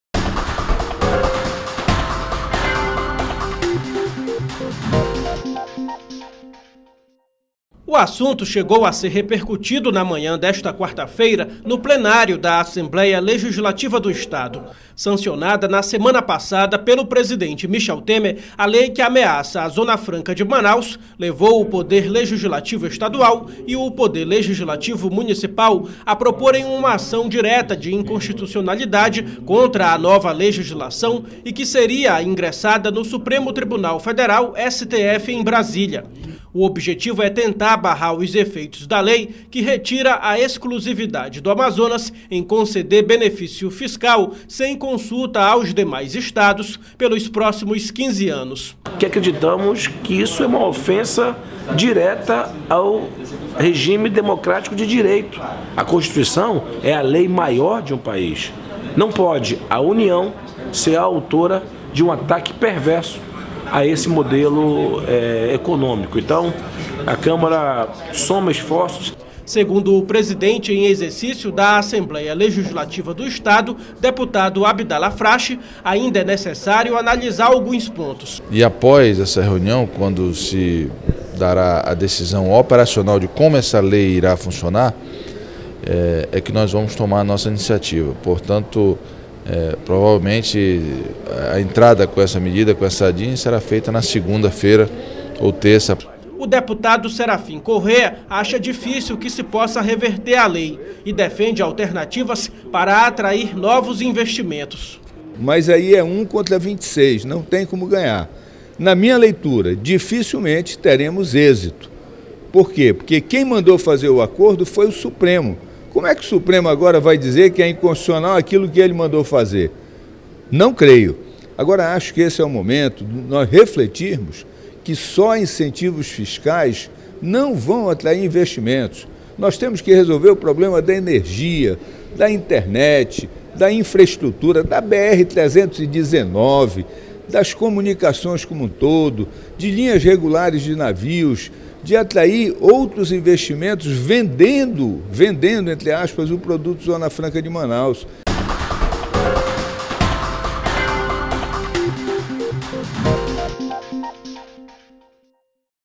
Lei que prejudica Zona Franca de Manaus deve ser julgada no STF
A lei sancionada pelo Governo Federal, que promete acabar com a guerra fiscal em 15 anos, mas que prejudica a Zona Franca de Manaus, deve ser alvo de uma ação no Supremo Tribunal Federal (STF). A alegação é que a nova lei seria inconstitucional.